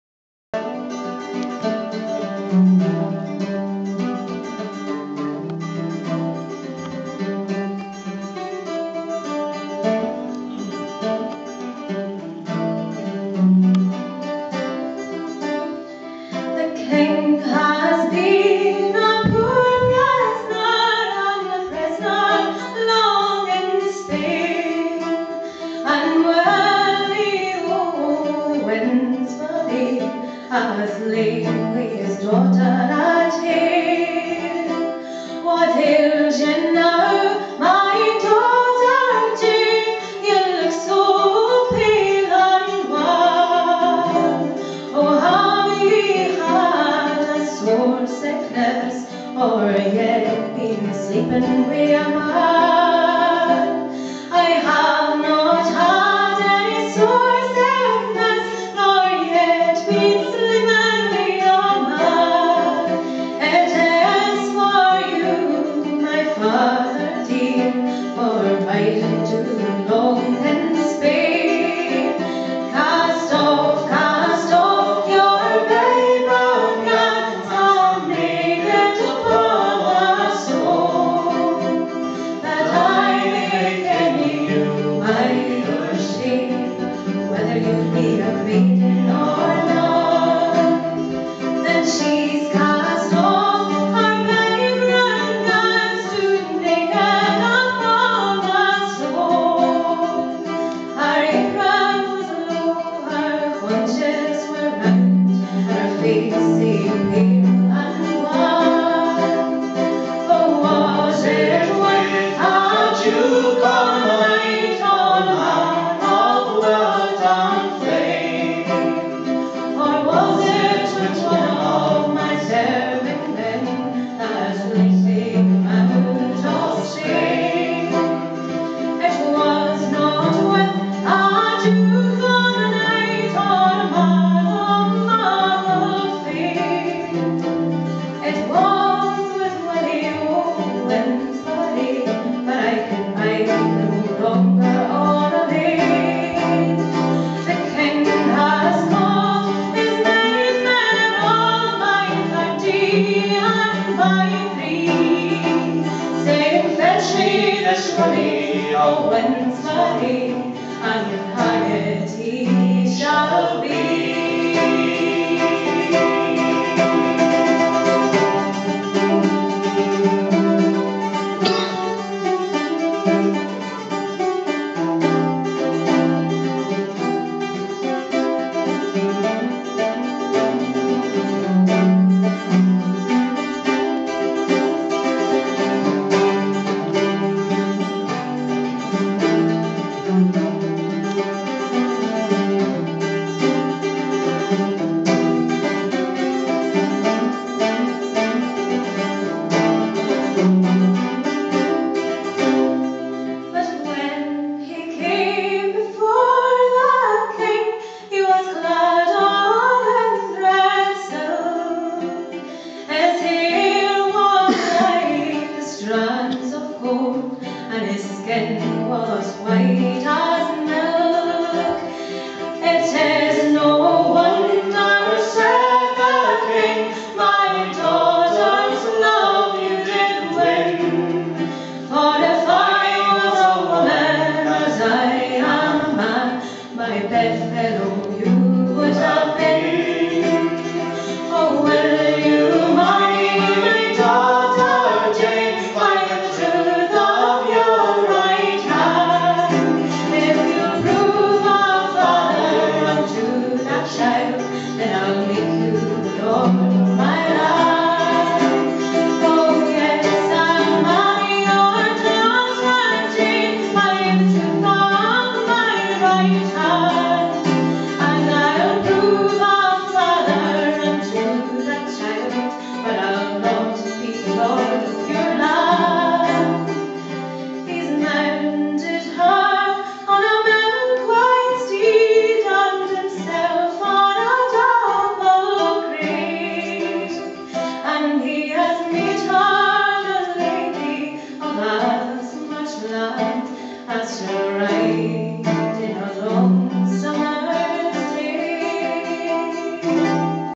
Traditional music concert by UL students: Robyn's
performed by Robyn Stapleton and Paahto Cummins
Traditional music concert by UL students, University of Limerick, Ireland